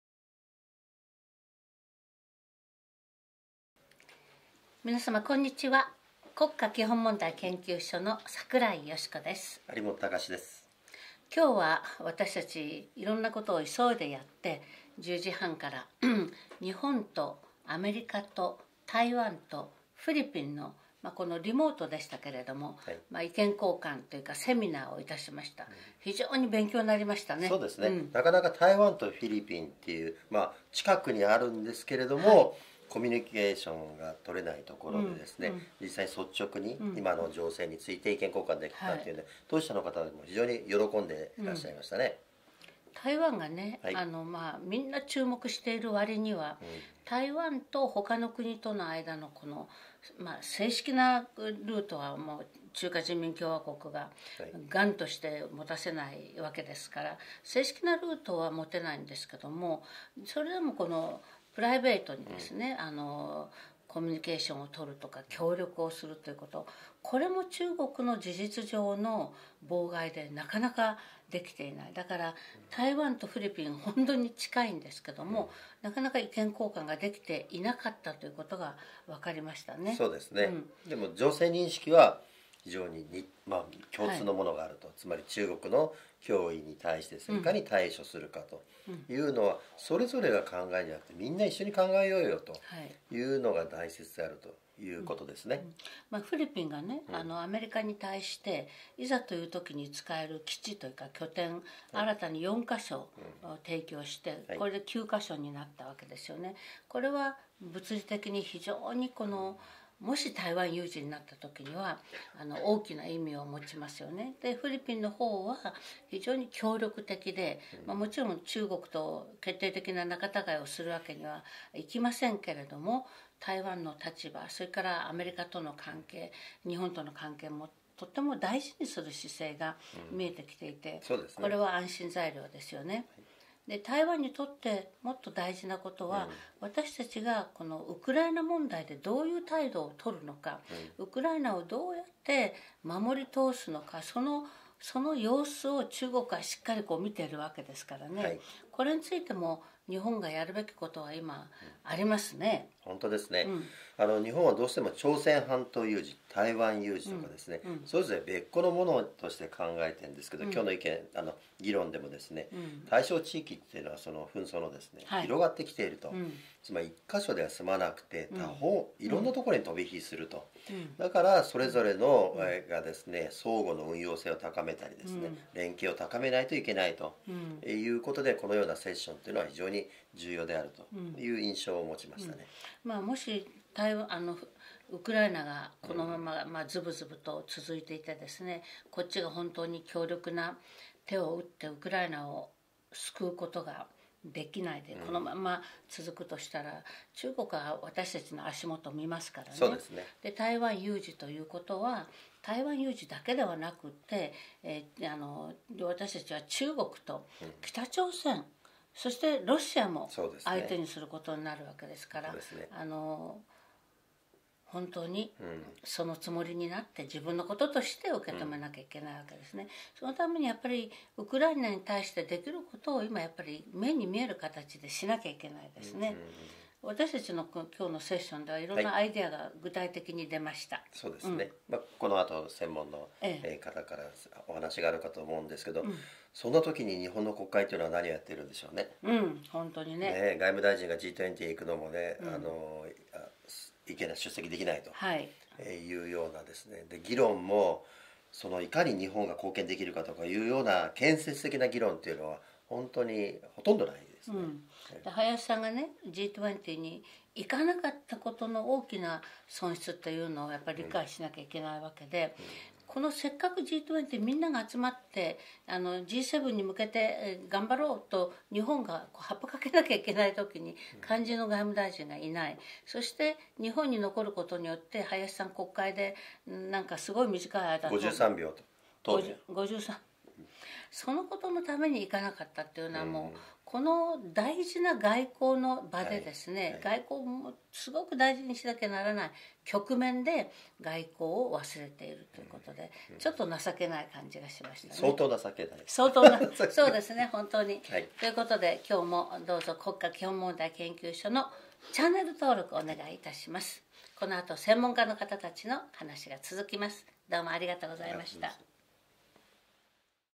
国基研チャンネル 第324回 日台米比でリモートセミナーを実施、日本の外相がG20不参加という失態。
『国基研チャンネル』第324回は櫻井よしこ理事長と有元隆志さん。本日は日台米比でリモートセミナーを実施。大陸の恫喝で注目が集まる台湾。4カ国で対話を重ねる意味は大きい。比で米軍が使える基地が増えたのは朗報。日本にとりウクライナ問題は次の台湾への試金石。しかし日本の外相がG20不参加という失態。もっと目に見える支援を積極的かつ速やかに。

櫻井よしこ　国基研理事長
有元隆志　国基研企画委員